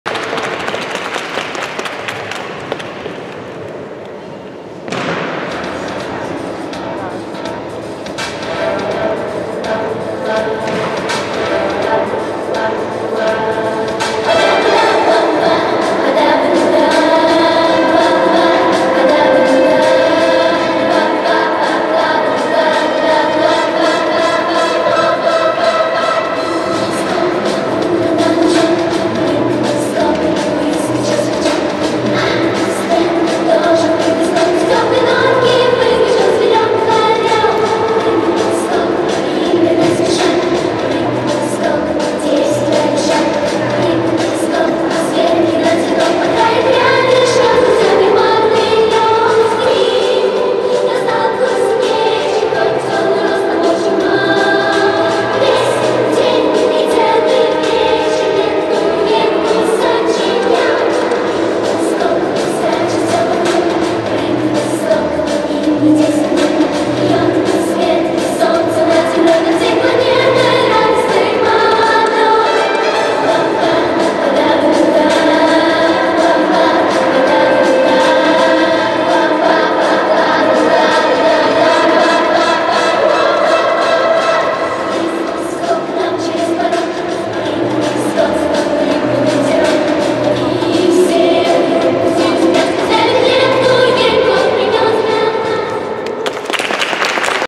Sofia Spiridonova (Excite Dance VK) vaba kava @Narva KV iluvoimlemises 2014
Klass : mini (2007)